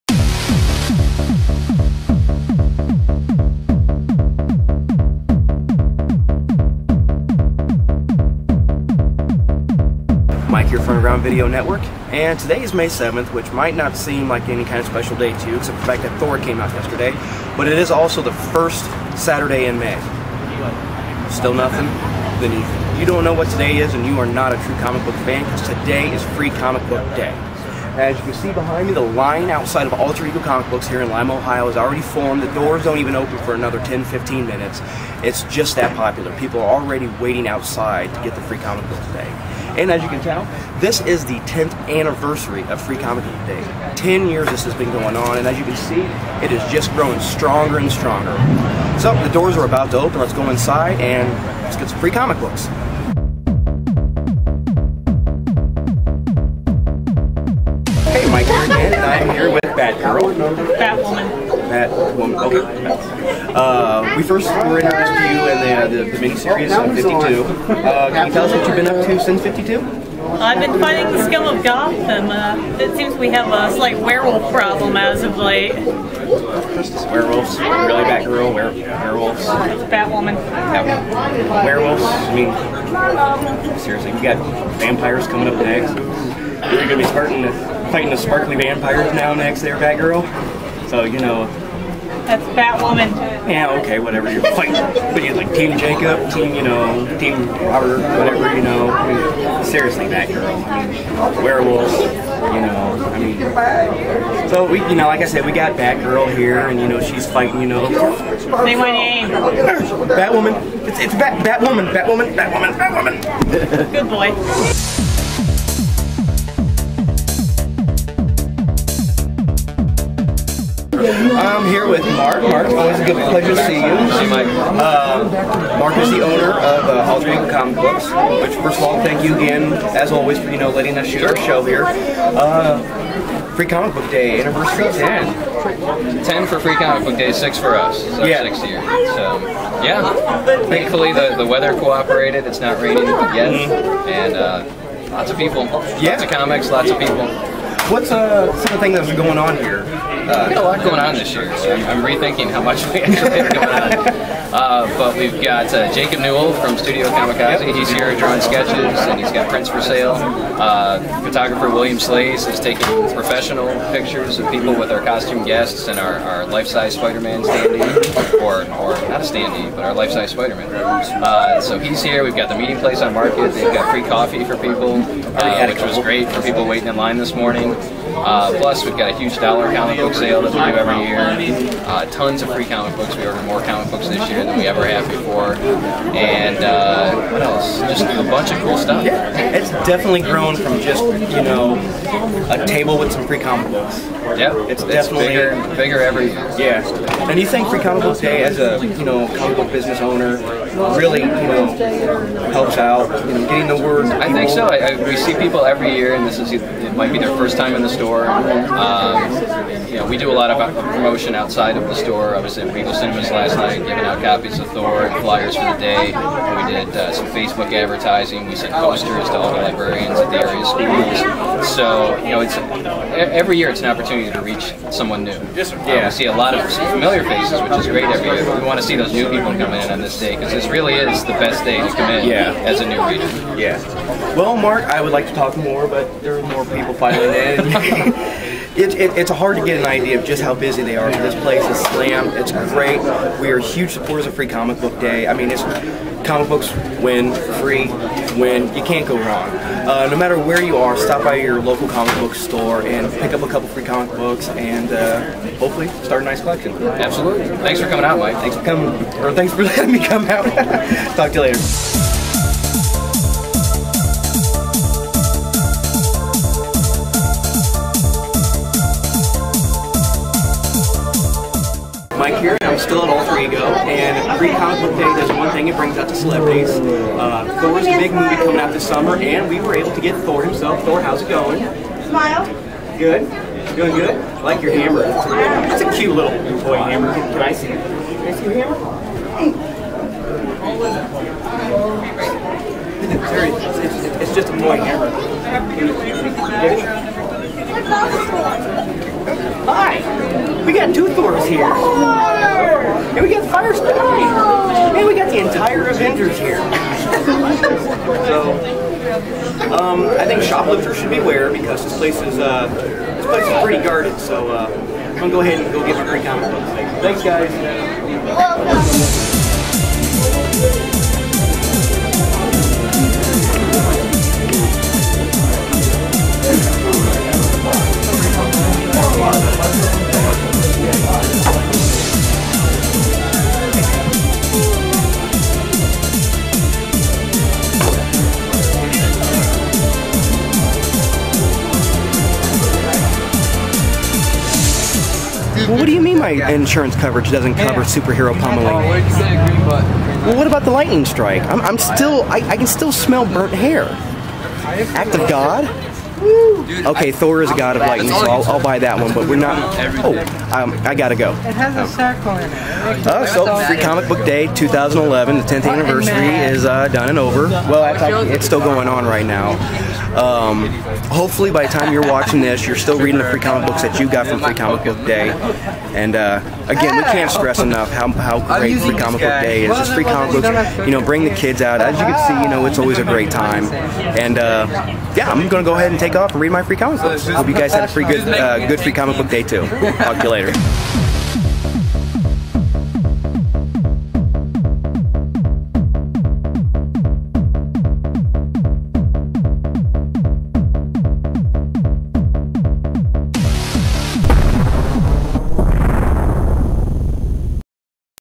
Free Comic Book Day 2011 at Alter Ego Comics
Mike's coverage of Free Comic Book Day at Alter Ego Comics (Lima, Ohio)